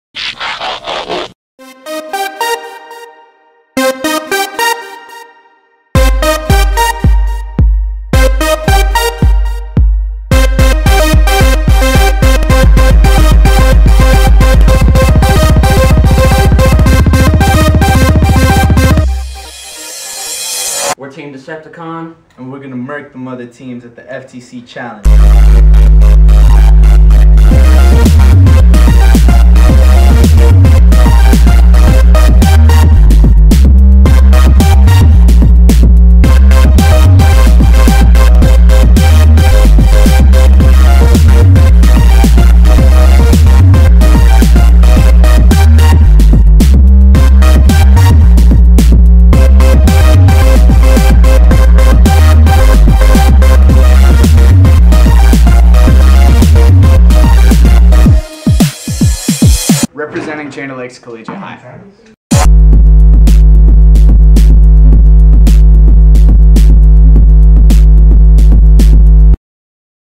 Decepticon Team 5232 (2011)
Chain of Lakes Collegiate High School students are performing as a rookie team in the FTC Robotic Competition.